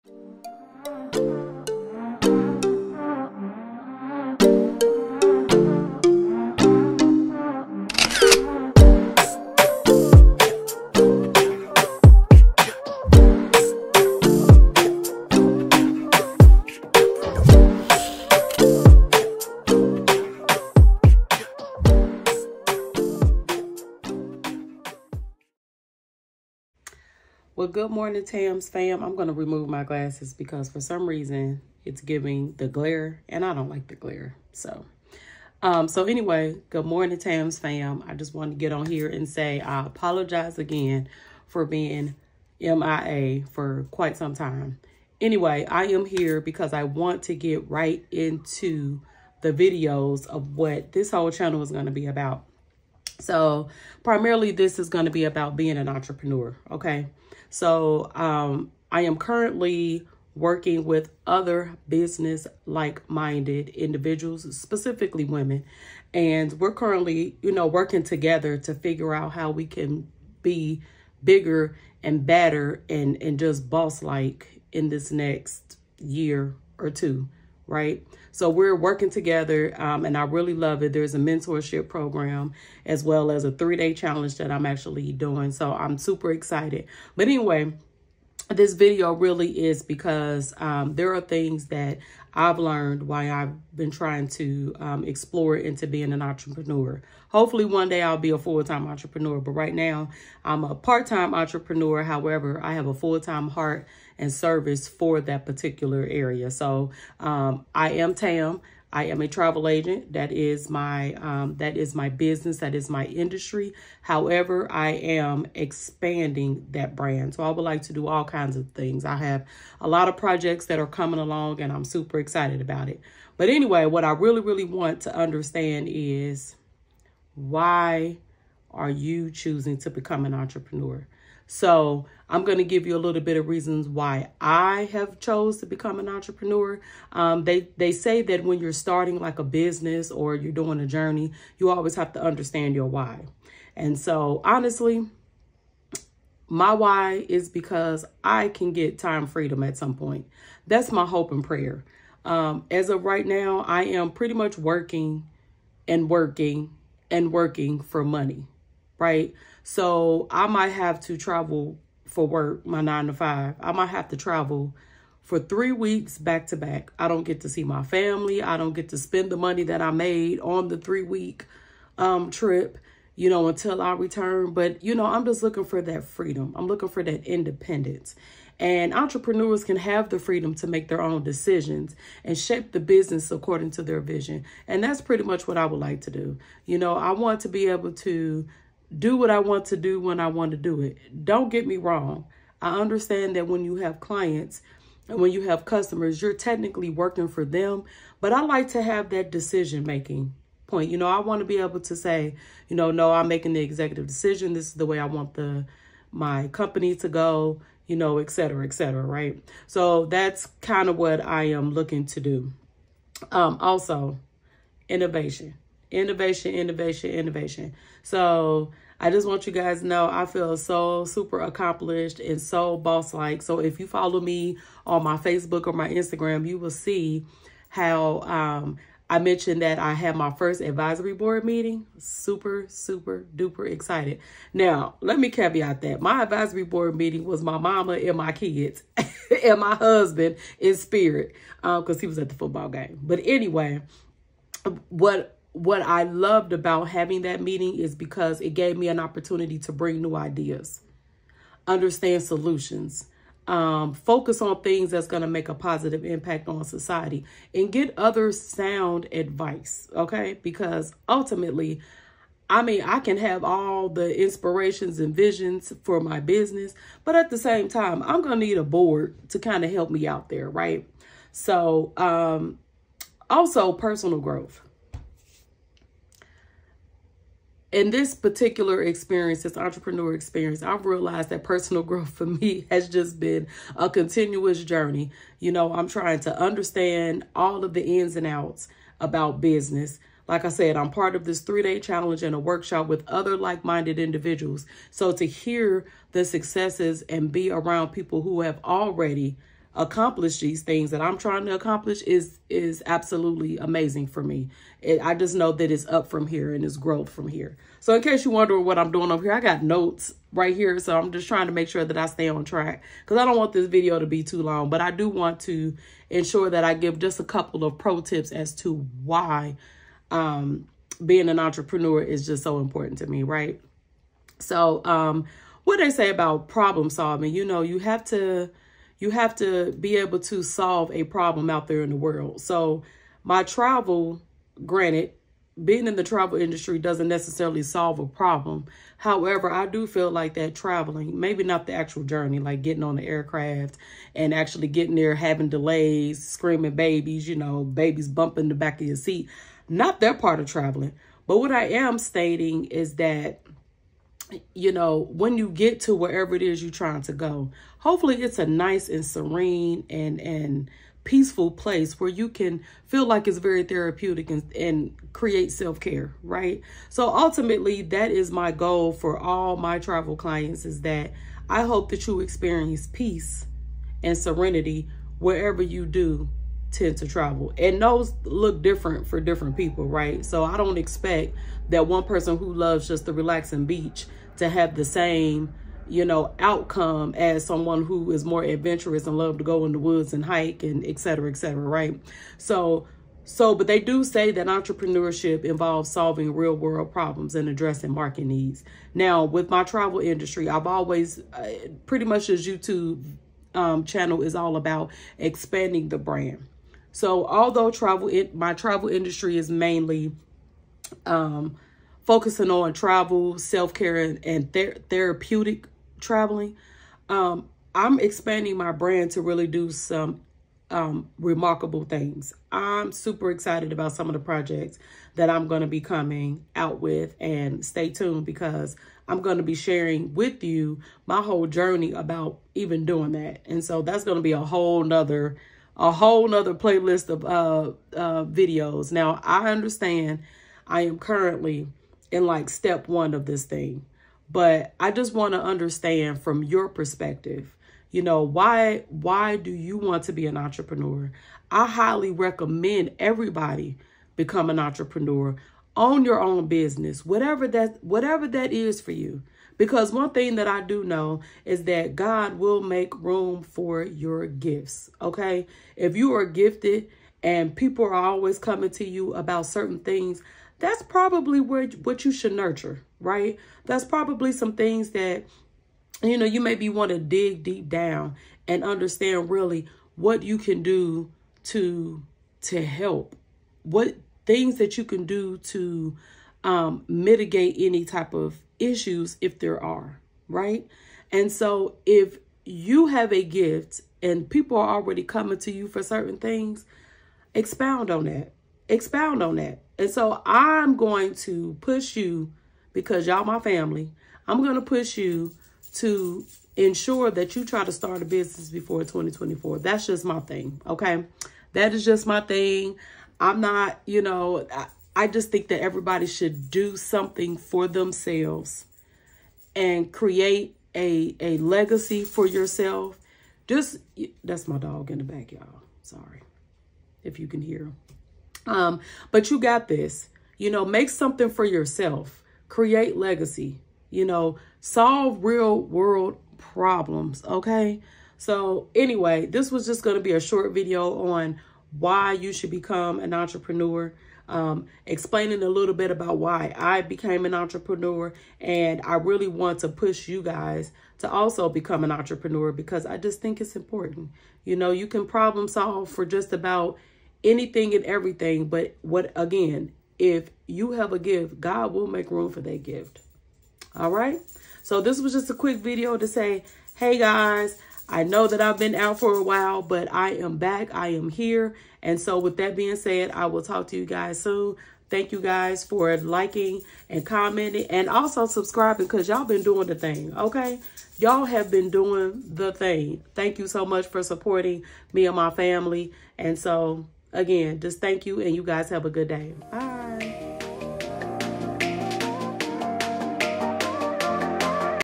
COFFEE TIME| Why YOU Should Start a Business NOW!!!
In this video I'm talking about tips on becoming a business owner and entrepreneur. I hope this video inspires you to become a higher and best self. I hope you enjoyed this video, Thank you Tam's Fam for all the love and support !!
Don't Forget to:
SHARE to Family and Friends